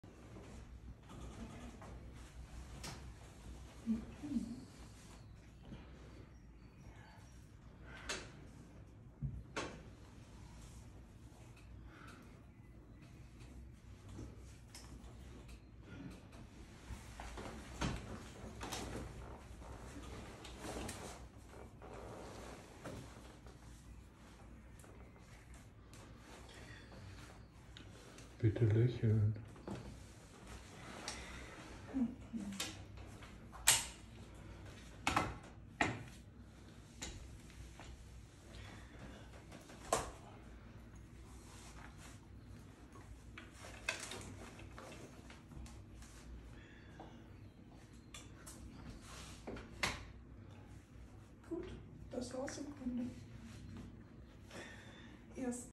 Knietransfer mit Männerkraft
Mit Männerkraft sieht manches anders aus und trotzdem: der Knietransfer Bett-Rollstuhl kann mit guter Technik ganz easy sein.